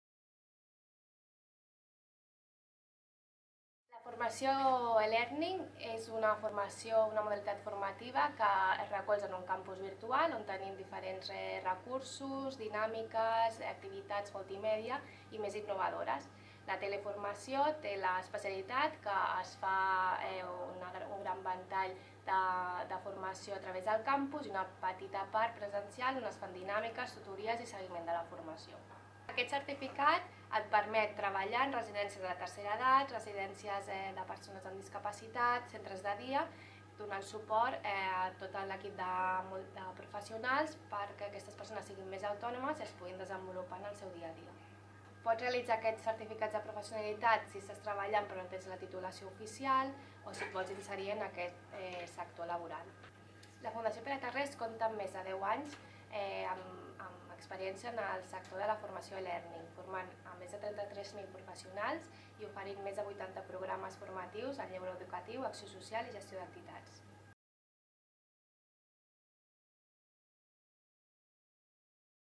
Certificat de professionalitat e-learning - Atenció sociosanitària en institucions socials
En aquest vídeo, la cap de Formació e-learning, Marta Vendrell, ens explica com s'impartirà aquesta nova modalitat de formació i quins avantatges tindrà respecte altres maneres de cursar aquest Certificat de Professionalitat